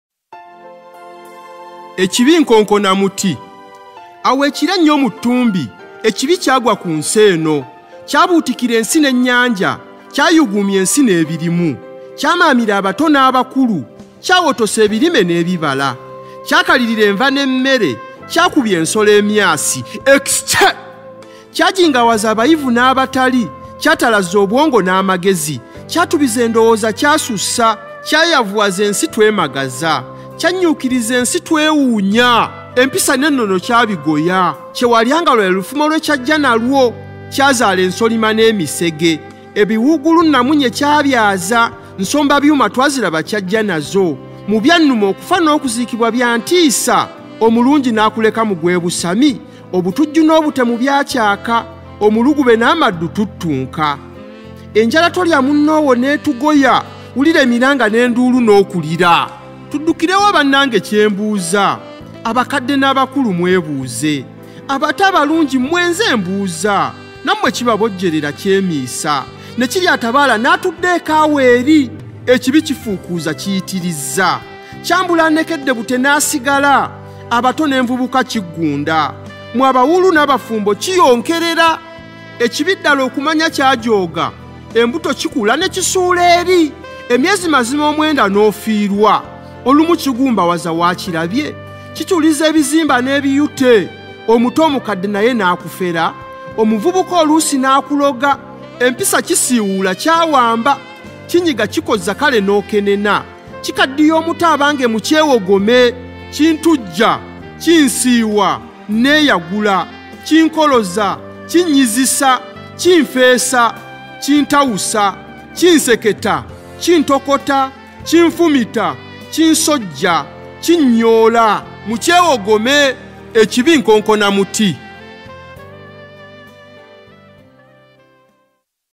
EKIBI NKONKONA MUTI- The Chronicle SIN
A thought-provoking exploration of the timeless impact of 'SIN' on human behavior throughout history. Delve into its evolution and ongoing influence across generations, shaping our world from the past to the future."